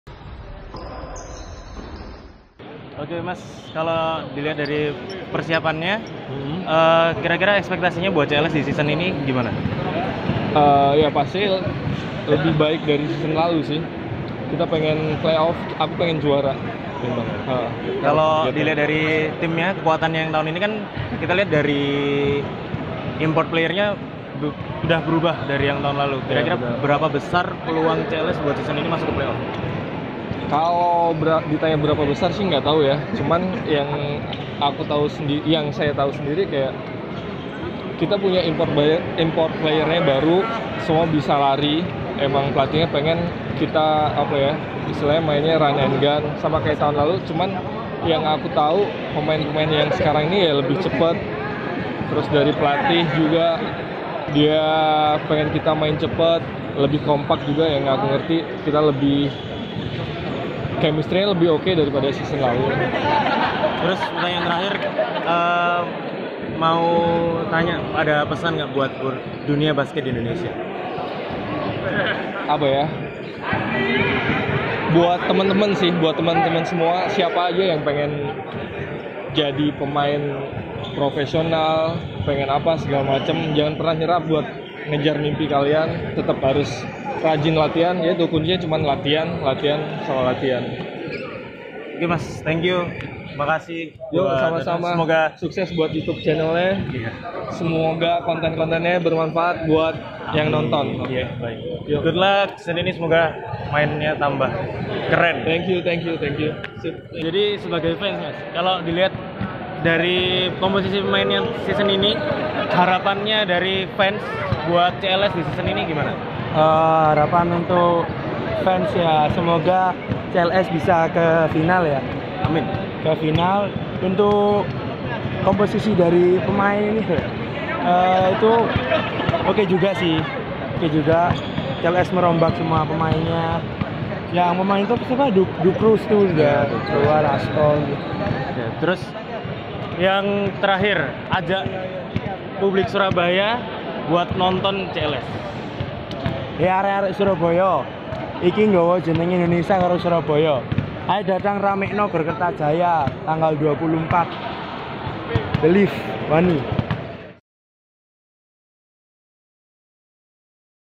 After Practice Interview #4 - Arif Hidayat & Defandra (Fans CLS Knights)
Kami berkesempatan untuk melakukan interview dengan Arif Hidayat dan Defandra (Fans CLS Knights) setelah Open Practice dan Team Launching yang diadakan oleh CLS Knights pada hari Rabu, tanggal 7 November 2018. Enjoy our interview with these two great players from CLS Knights!